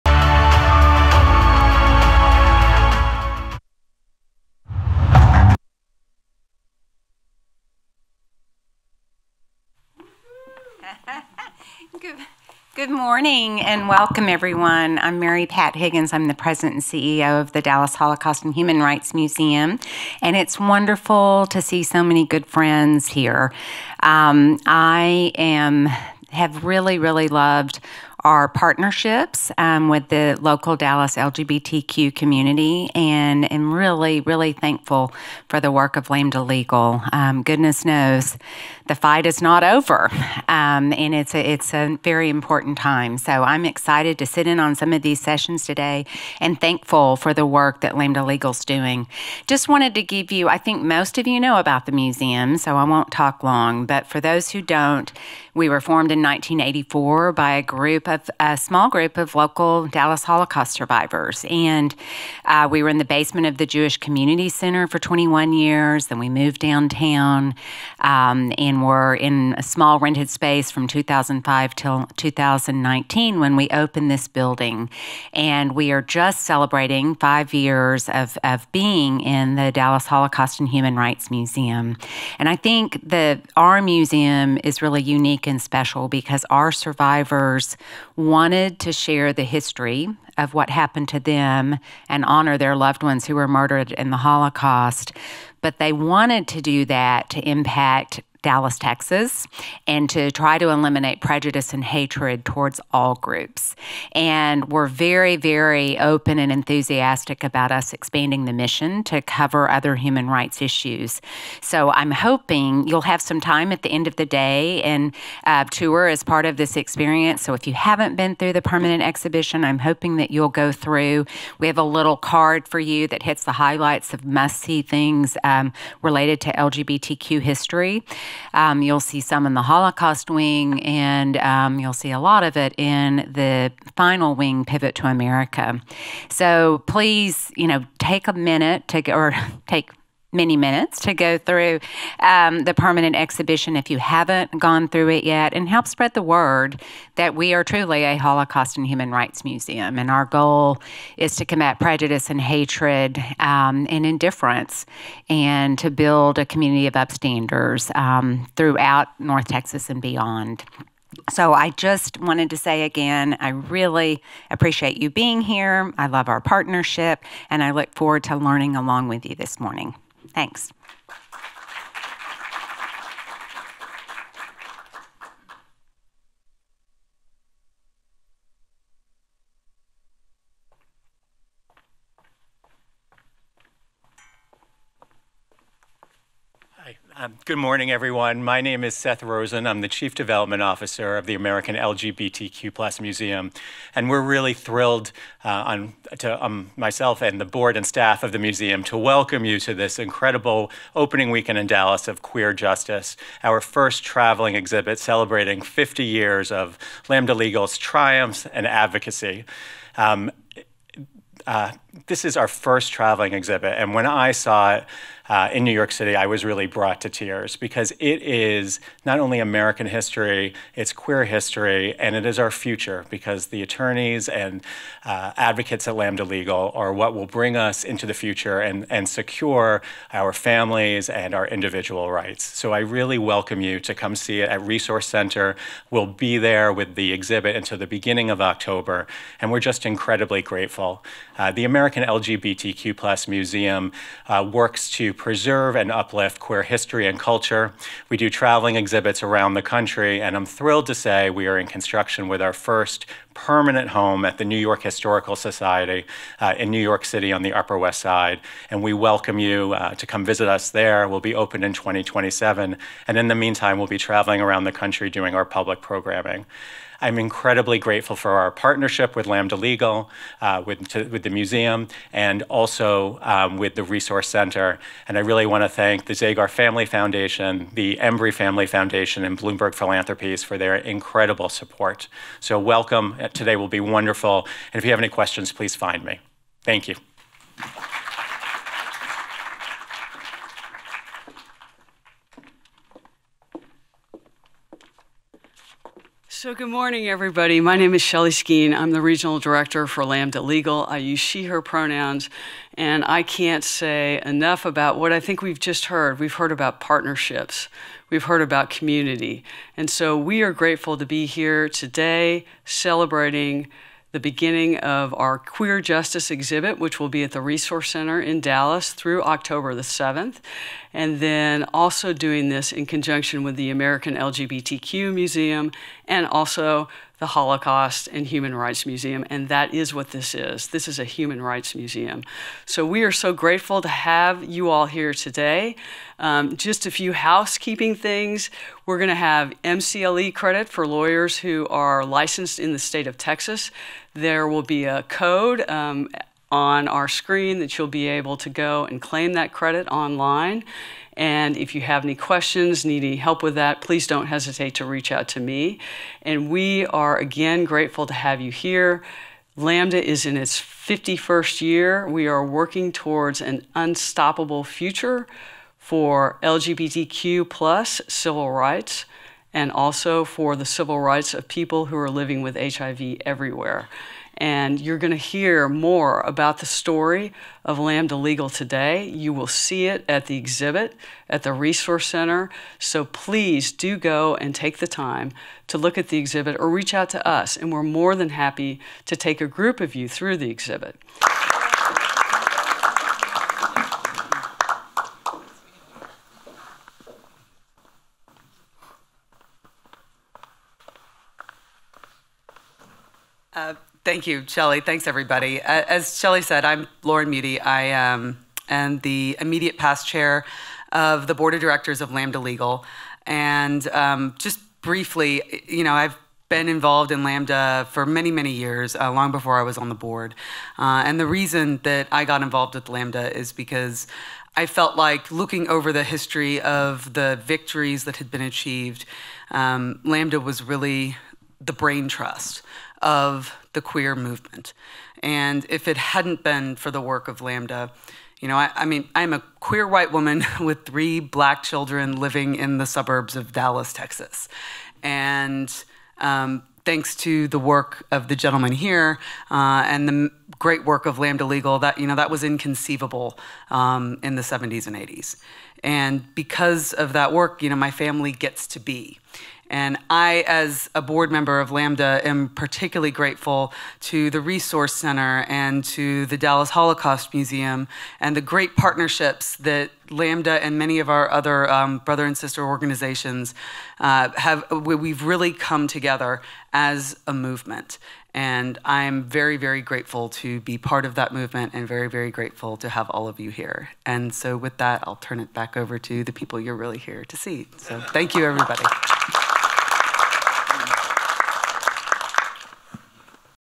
The Unstoppable State of LGBTQ+ Rights: Past, Present, and Future: Introduction
Created in collaboration with the American LGBTQ+ Museum, Queer Justice celebrates 50 years of Lambda Legal’s crucial work defending the rights of the LGBTQ+ community and everyone living with HIV. We were proud to welcome folks for this powerful symposium on The Unstoppable State of LGBTQ+ Rights: Past, Present, and Future at the Dallas Holocaust and Human Rights Museum.